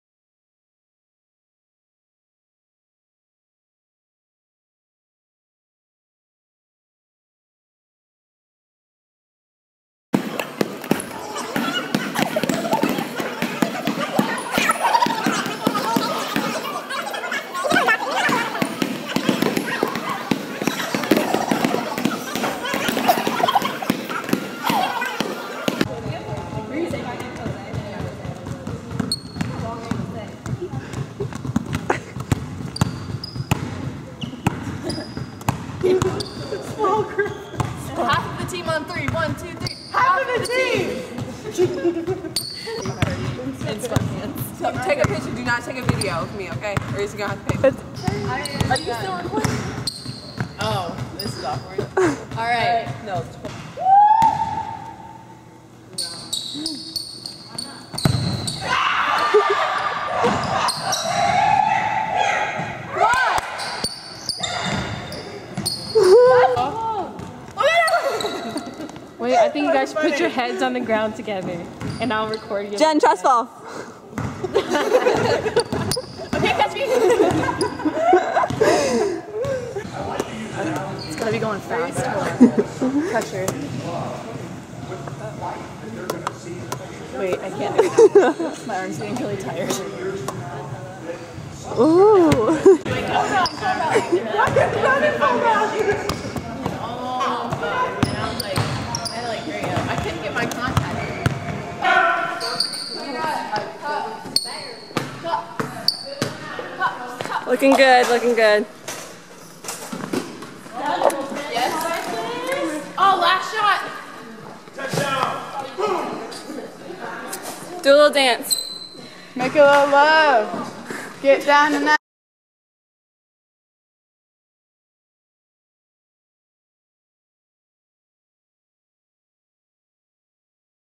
Team Cohesiveness Bloopers
My teammates act silly when cameras are around